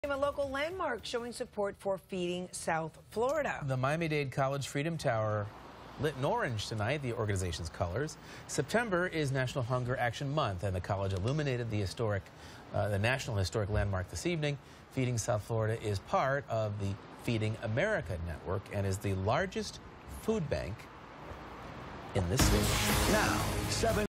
Miami Dade College’s Freedom Tower Goes Orange for Hunger Action Day 2016
A local historic landmark showed support for Feeding South Florida, Thursday night in honor of Hunger Action Day.

Miami Dade College’s Freedom Tower was lit up in orange at sunset, the color of Feeding South Florida, in observance of National Hunger Action Month.